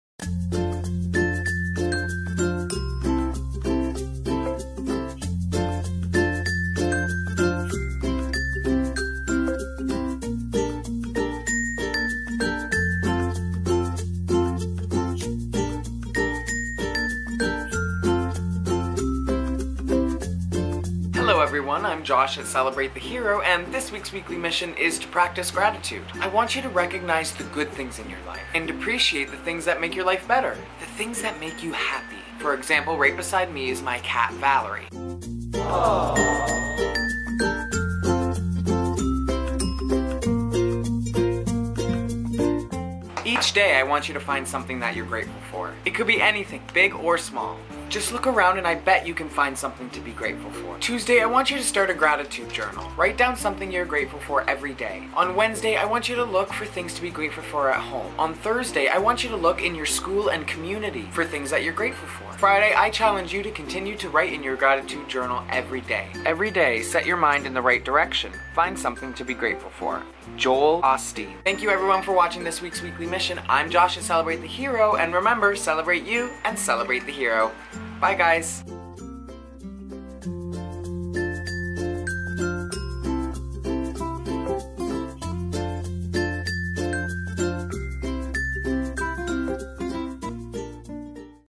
Weekly Mission: Practice Gratitude
Weekly Mission:
Practice Gratitude

This week’s mission is Practice Gratitude. We want you to recognize the good things in your life, and appreciate the things that make your life better, the things that make you happy. Each day, we want you to find things to be grateful for. It could be anything, big or small. Just look around and you can find things to be grateful for.

Tuesday: Start a Gratitude Journal. Write down something you are grateful for every day.

Wednesday: Today, look for things to be grateful for at home.

Thursday: Today, look for things to be grateful for in your school or community.

Friday: We challenge you to continue writing in your Gratitude Journal every day.

“Every day, set your mind in the right direction. Find something to be grateful for” – Joel Osteen

Celebrate You, Celebrate the Hero!